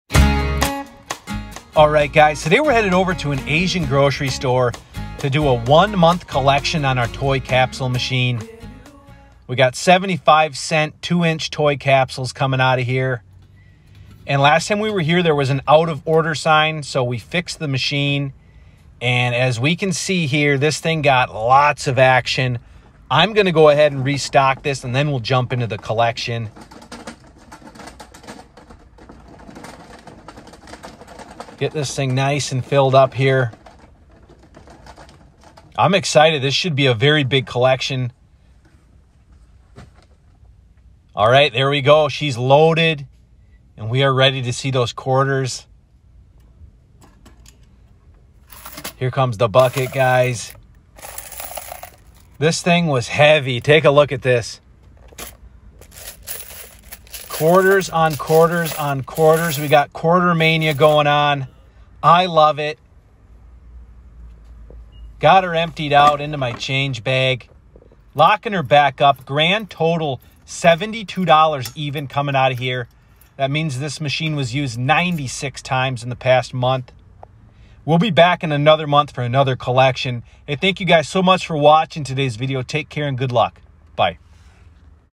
BIG 1 Month Vending Collection🤑💰TOY Capsule Machine!
Today we stop at an Asian grocery store to do a one month collection on our Toy Capsule machine!

Toplinethriftfind